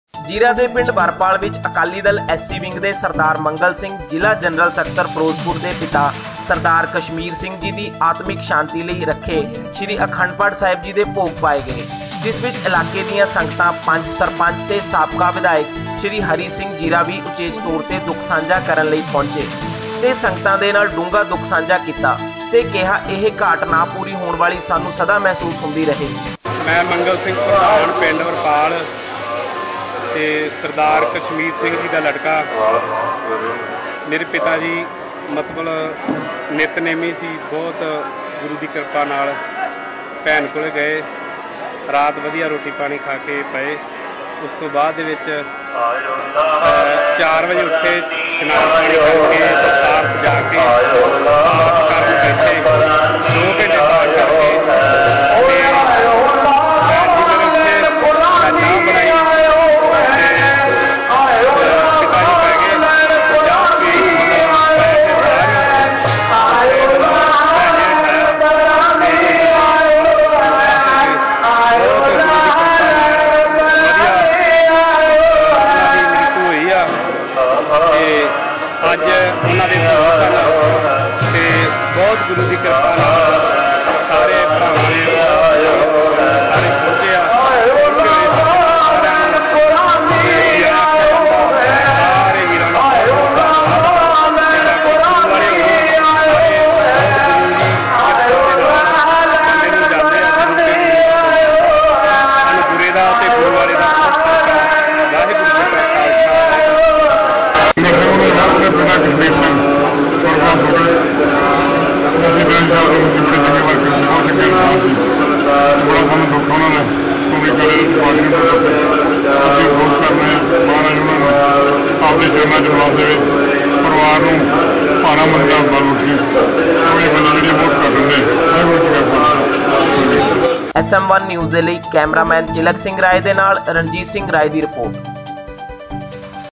ਸਾਬਕਾ ਵਿਧਾਇਕ ਵੱਲੋ ਜਨਰਲ ਸਕੱਤਰ ਅਕਾਲੀ ਦਲ ਐਸ ਸੀ ਦੇ ਪਿਤਾ ਦੀ ਮੌਤ ਤੇ ਦੁੱਖ ਸਾਂਝਾ
sm1news